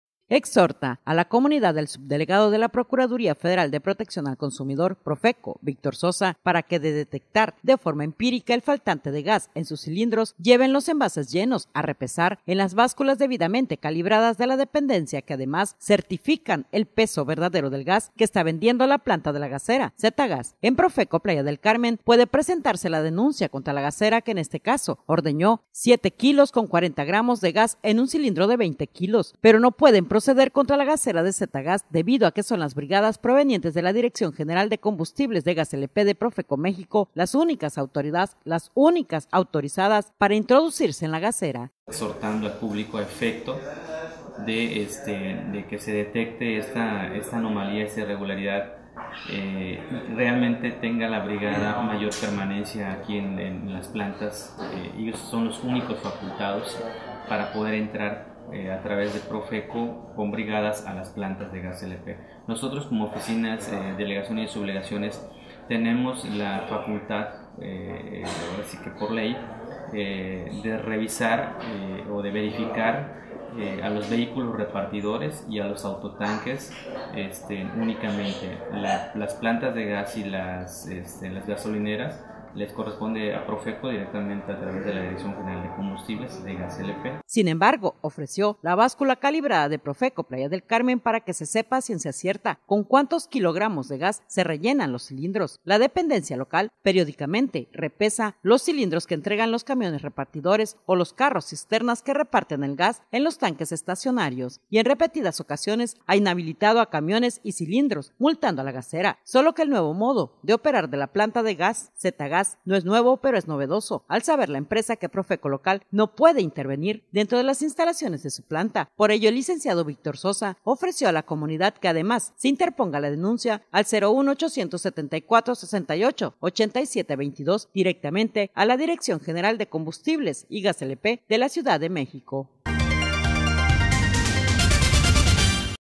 PROFECO exhorta a denunciar a gasera
Playa del Carmen, Q. Roo.- Exhorta a la comunidad, el Subdelegado de la Procuraduría Federal de Protección al Consumidor (Profeco) Victor Sosa, para que de detectar de forma empírica el faltante de gas en sus cilindros, lleven los envases llenos a repesar en las básculas debidamente calibradas de la dependencia que además certifican el peso verdadero del gas que está vendiendo la planta de la gasera "Z-Gas".